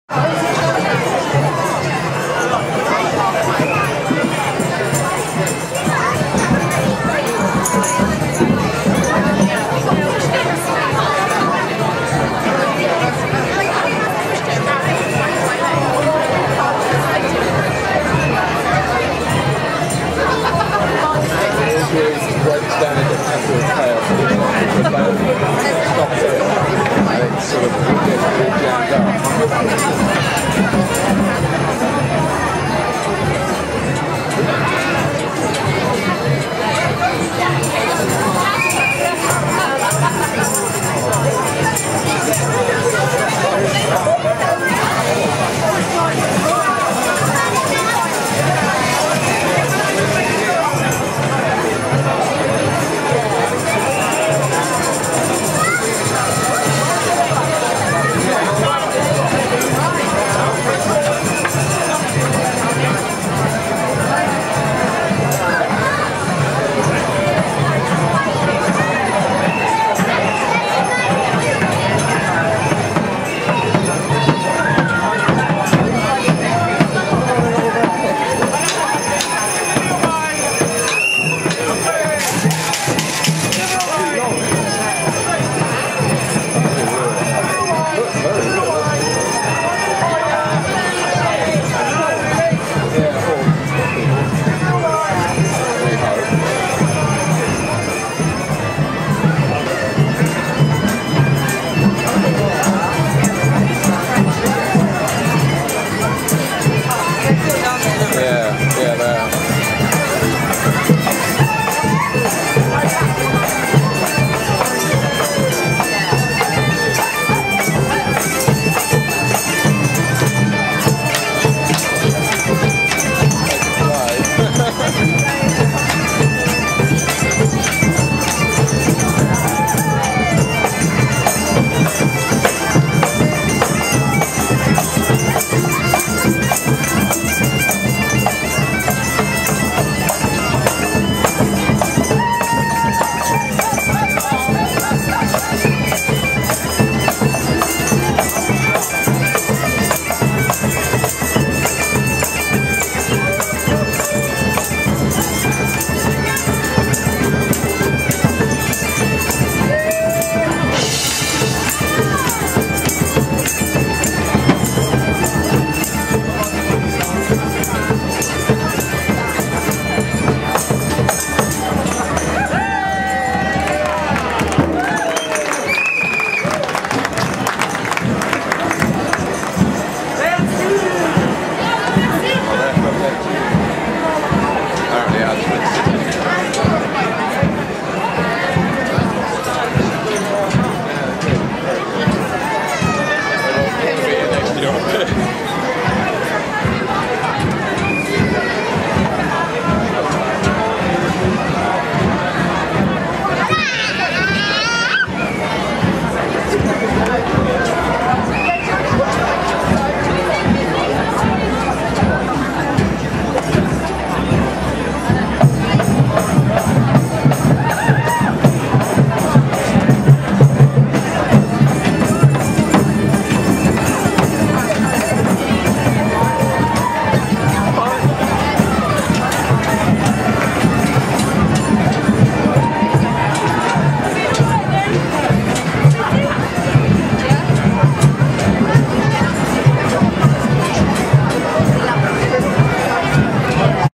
Hastings Jack in the green
May day in Hastings out side our shop. We sell books and at night Thai food. So you may enjoy a meal and browse our book stock.